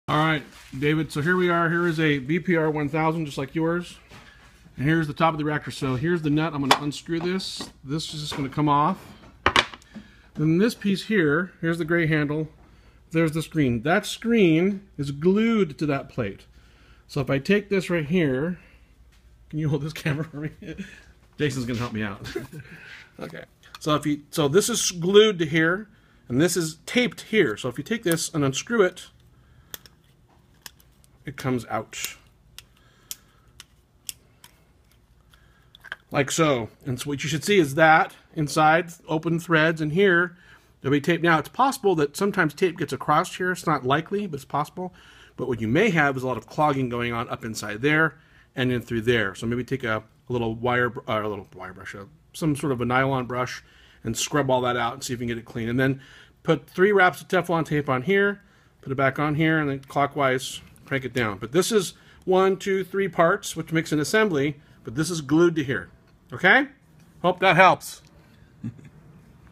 CRAS Presents... How to remove the outlet valve from your Bio-Pellet Reactor
In this short video I illustrate how to remove the outlet valve from a bio-pellet reactor so that it can be cleaned and cleared of any debris they may be impeding flow.